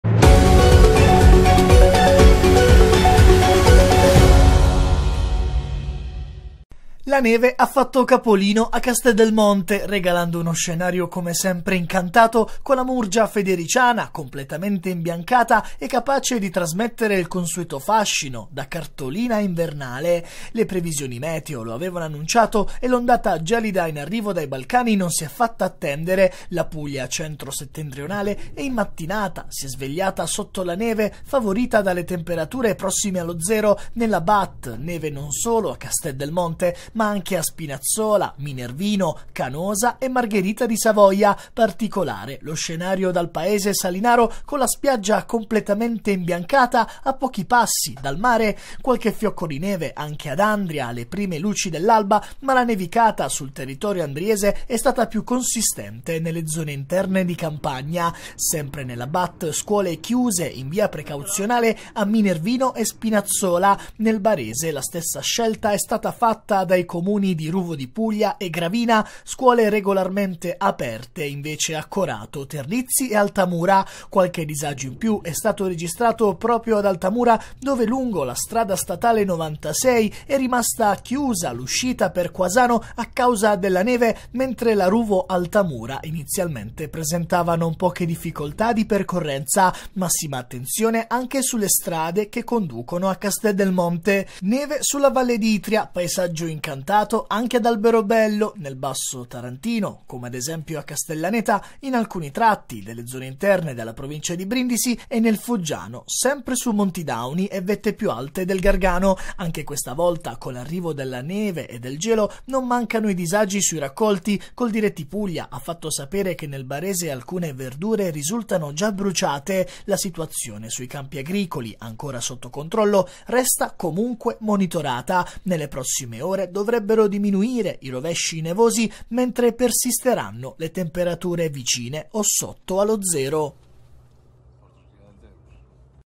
PUGLIA | Risveglio sotto la neve a centro-nord, anche Castel del Monte imbiancato
Scuole chiuse a Gravina, Ruvo, Minervino e Spinazzola